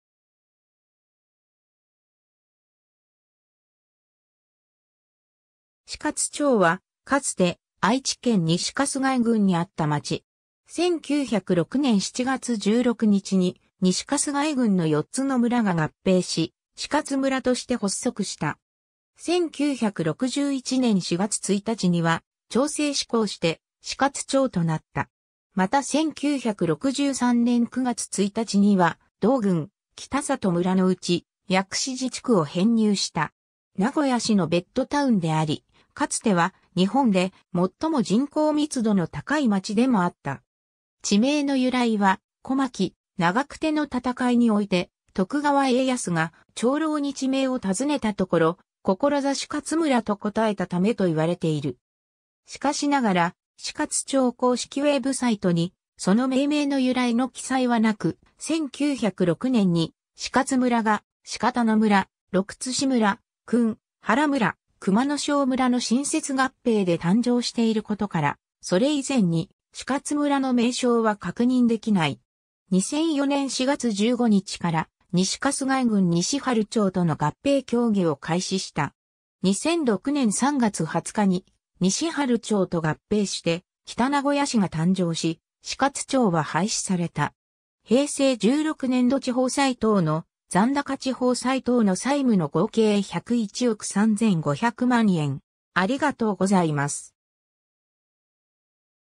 師勝町
師勝町（しかつちょう）は、かつて愛知県西春日井郡にあった町。
 1906年（明治39年）7月16日に西春日井郡の4つの村（訓原村、鹿田村、六ツ師村、熊之庄村）が合併し、師勝村として発足した。
1961年（昭和36年）4月1日には町制施行して師勝町となった。
また1963年（昭和38年）9月1日には、同郡北里村のうち薬師寺地区を編入した。
名古屋市のベッドタウンであり、かつては「日本で最も人口密度の高い町」でもあった。
 地名の由来は、小牧・長久手の戦いにおいて、徳川家康が長老に地名を尋ねたところ「志勝村」と答えたためと言われている。
しかしながら、師勝町公式ウェブサイトにその命名の由来の記載はなく、1906年に師勝村が、鹿田村、六ツ師村、訓原村、熊之庄村の新設合併で誕生していることから、それ以前に師勝村の名称は確認できない。
 2004年（平成16年）4月15日から西春日井郡西春町との合併協議を開始した。
2006年（平成18年）3月20日に西春町と合併して北名古屋市が誕生し、師勝町は廃止された。
 平成16年度（2004年度） 地方債等の残高 地方債等の債務の合計 101億3500万円 （連結会計の考えに基づく　ただし特別会計分の地方債は一切含まず）